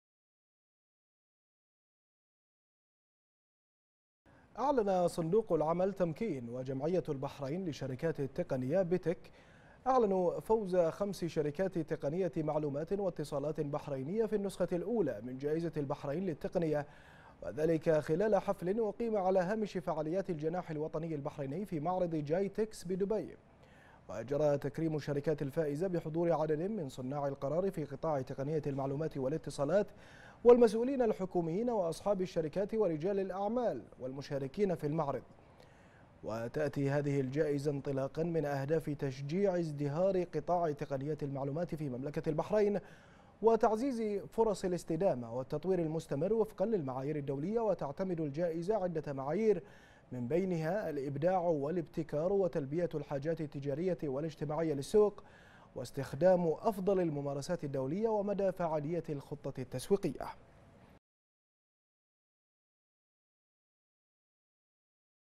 البحرين : تمكين و "بتك" تعلنان فوز 5 شركات بحرينية بجائزة البحرين للتقنية
تمكين و "بتك" تعلنان فوز 5 شركات بحرينية بجائزة البحرين للتقنية